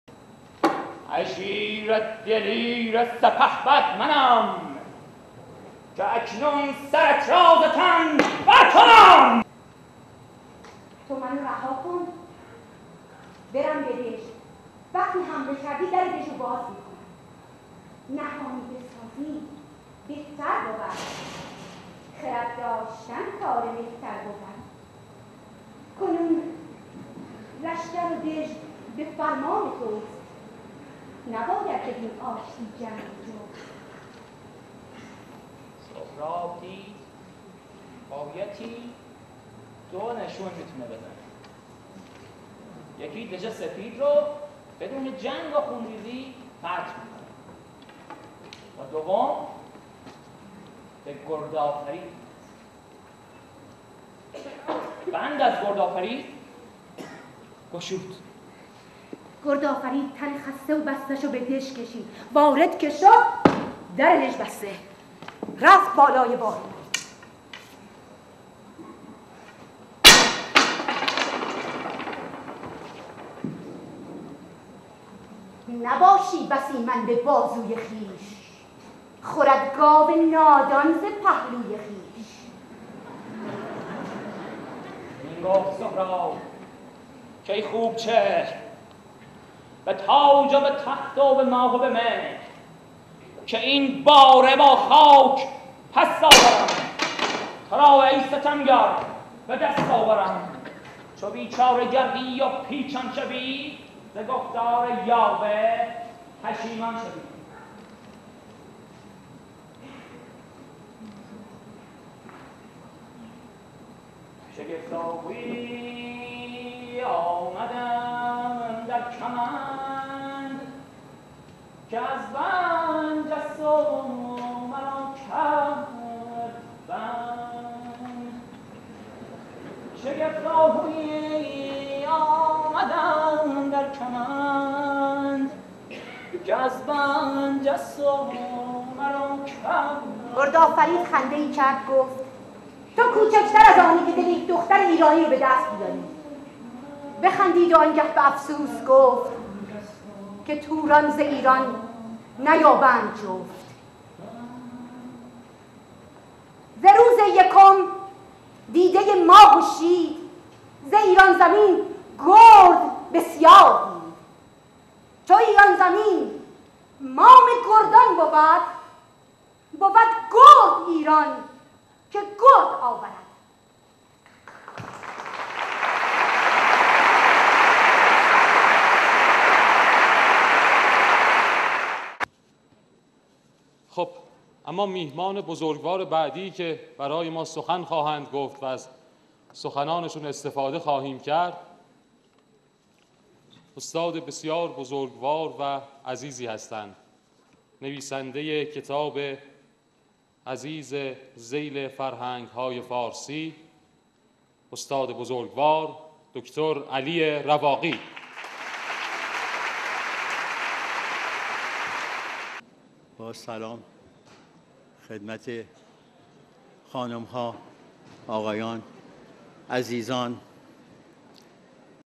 نبرد سهراب با دخت دلاور ایران زمین، دانشگاه تهران همراه با پژوهندگان شاهنامه و اساتید موسیقی
نقال ها: مرشد عقیلی و آرش فرهادی